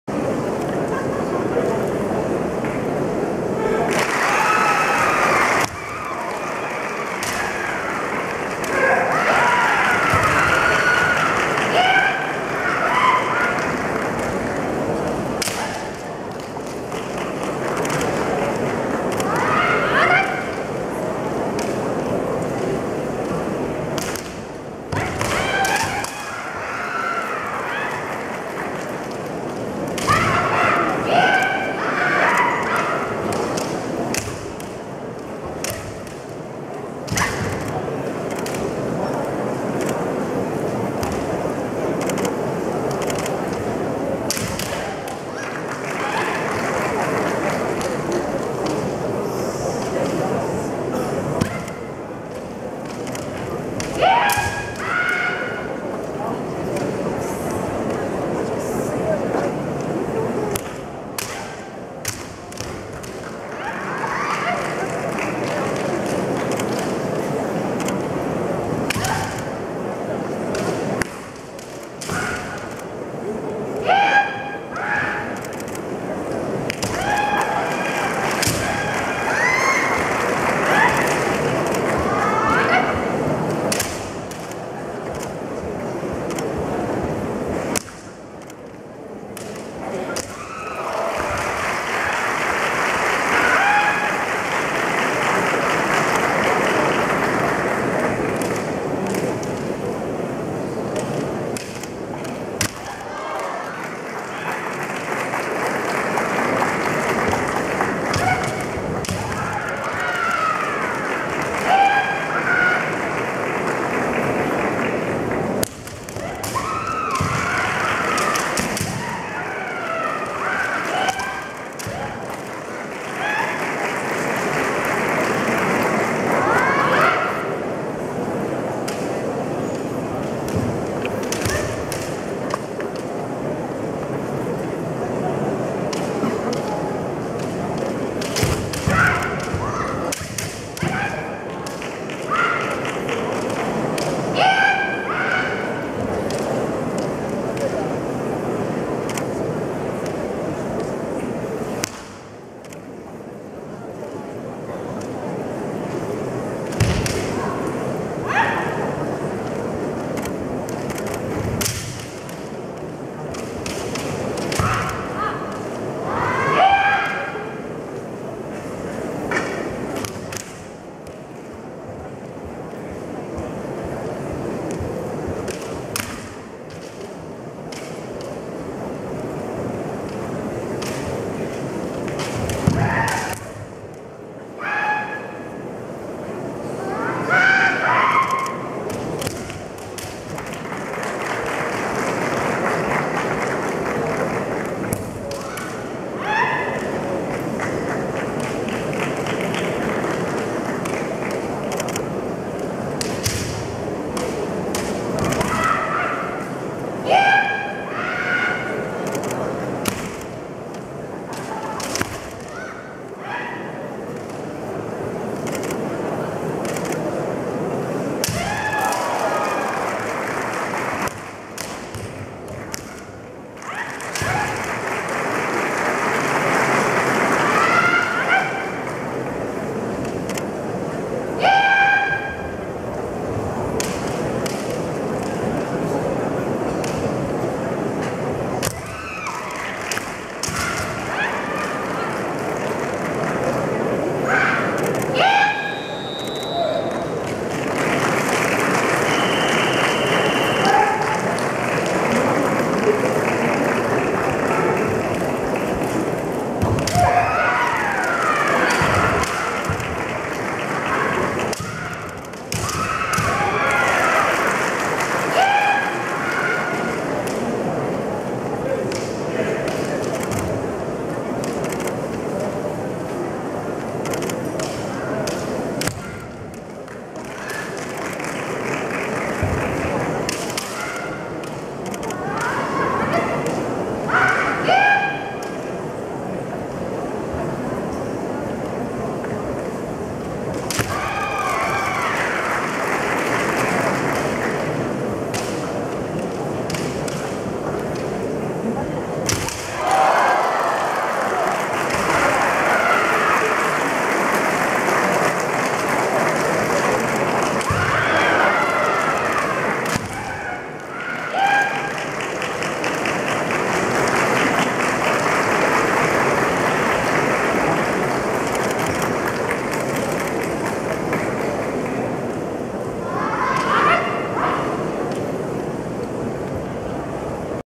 H27関東女子学生剣道選手権 準決勝1 田村(國士舘)対佐々木(筑波)
H27/5/16、H27第47回関東学生剣道選手権大会の結果・詳細は剣道総合サイトLET'S KENDOにて！！
【結果】
優勝 福川菜月（法政大学2年、八代白百合）

二位 佐々木奈緒（筑波大学4年、中村学園女子）

三位 田村久美（国士舘大学4年、桜ヶ丘）

三位 染谷千奈美（国士舘大学3年、流山）

・敢闘賞・ベスト8
和栗宏恵（駒澤3年、駒澤大）
寺岡留那（日大1年、日大）
三好絢女（明治3年、麗澤瑞浪）
鈴木瑞生（筑波4年、小山）